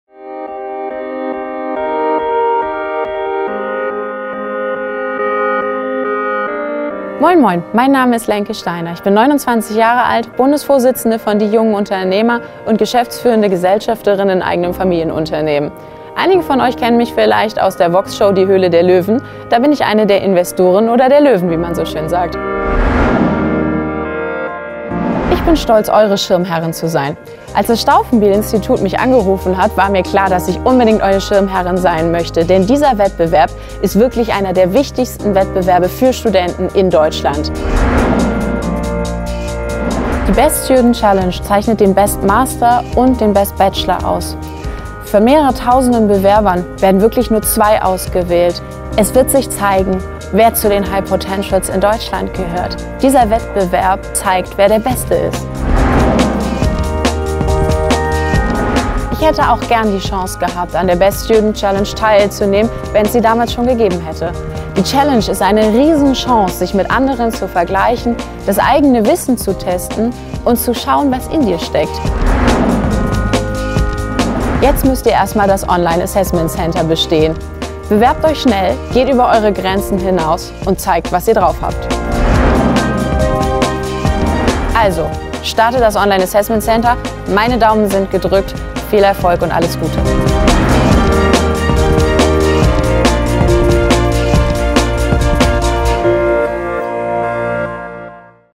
BestStudentChallenge 2015 - Die Schirmherrin Lencke Steiner
Ein Tag. Sechs Challenges. 60 Kandidaten – und die zwei besten erhalten 1.000 Euro Siegprämie. Die Best Student Challenge kürt am 11. März den „Best Master“ und „Best Bachelor“ Deutschlands.

W-Pack-Geschäftsführerin Lencke Steiner freut sich, die Schirmherrschaft für diesen besonderen Wettbewerb zu übernehmen. Die Bundesvorsitzende der Jungen Unternehmer ist u.a. auch Jury-Mitglied bei der Vox-Sendung „Die Höhle der Löwen“, einem Casting-Format für Existenzgründer, bei dem es gilt, die Juroren von der eigenen Geschäftsidee zu überzeugen.

In diesem kurzen Video erzählt sie, weshalb sie sehr gerne die Schirmherrschaft für die BSC2015 übernommen hat und was sie den Kandidaten mit auf den Weg geben möchte.